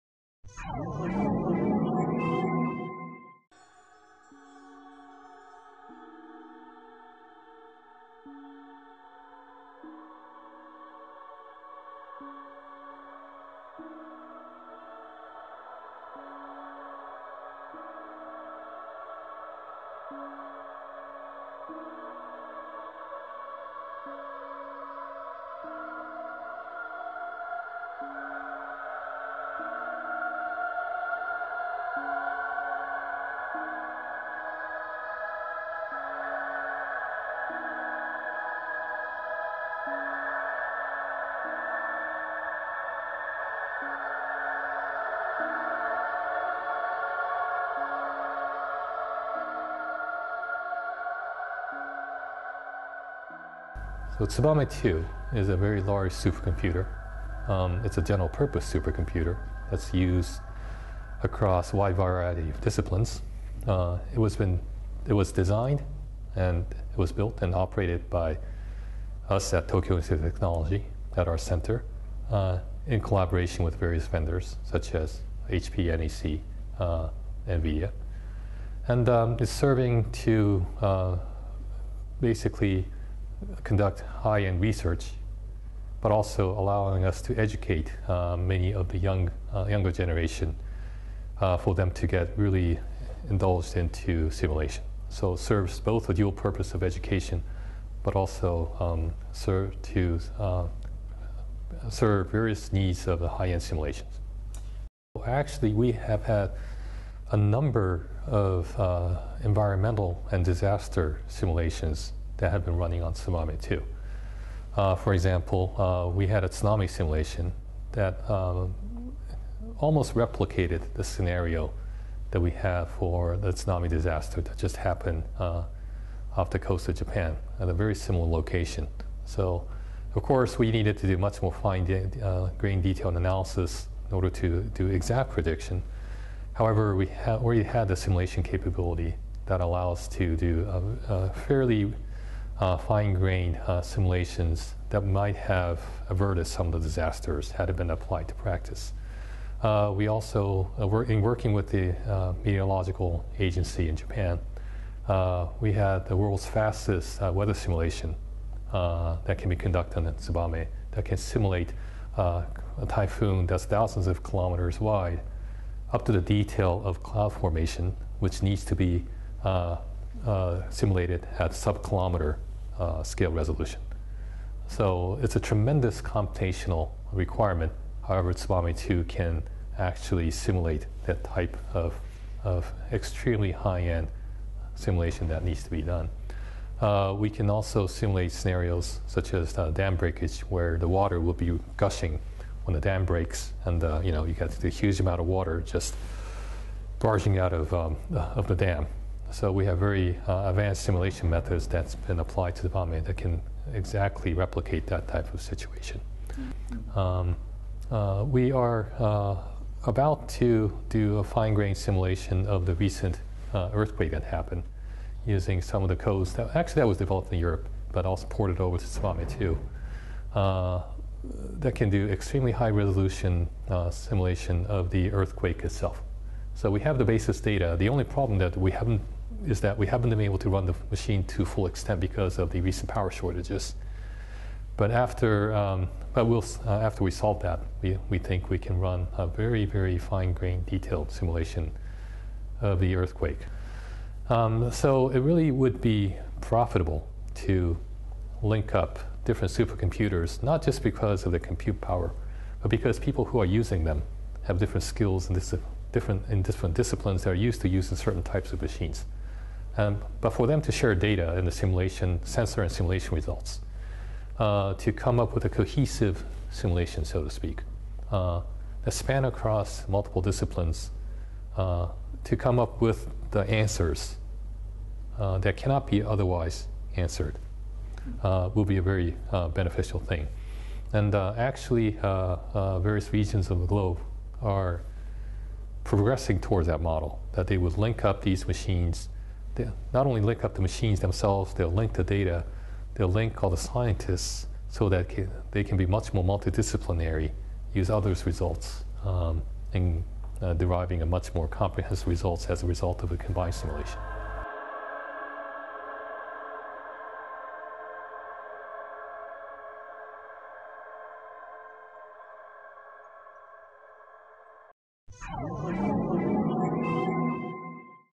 Supercomputer analyses Japan quake
Professor Satoshi Matsuoka speaks to Kris Sangani while visiting Bristol University as a guest lecturer and speaker. He explains how The Tokyo Institute of Technology's Tsubame 2.0 super computer will be used to analyse data gathered from the recent Quake and Tsuname in Japan